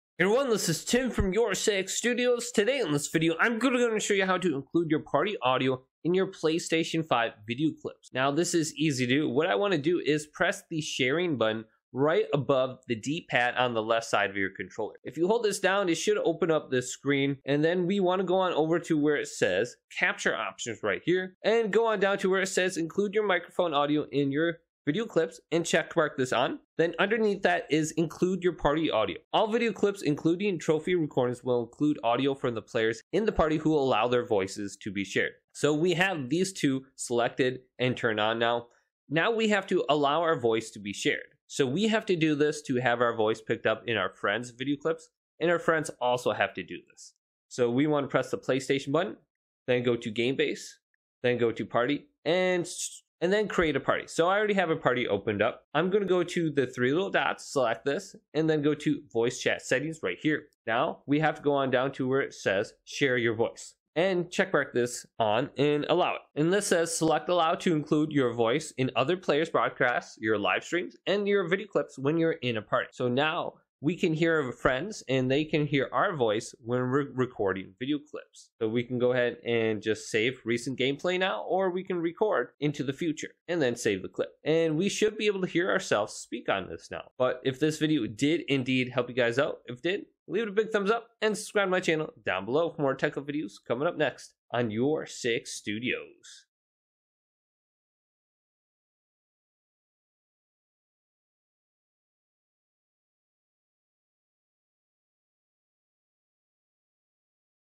How to Include Party Audio in PS5 Video Clips & Record Voices (Allow to SHARE)
To allow party chat audio in PS5 video clips, you can follow these steps:

Start your PS5 console and log in to your account.
Press the Create button on your DualSense controller to open the Create menu.

From the Create menu, select "Capture Gallery."
In the Capture Gallery, navigate to the video clip that you want to include party chat audio. Highlight the video clip and press the Options button on your controller.
From the options menu, select "Edit."

In the video clip editor, look for an option that allows you to adjust the audio settings. Within the audio settings, you should find an option to include party chat audio. Enable this option.

Once you have enabled the option to include party chat audio, review and confirm any other desired video editing settings.
After making the necessary adjustments, save the changes to the video clip.

Also allow you voice to be shared by going to the party settings!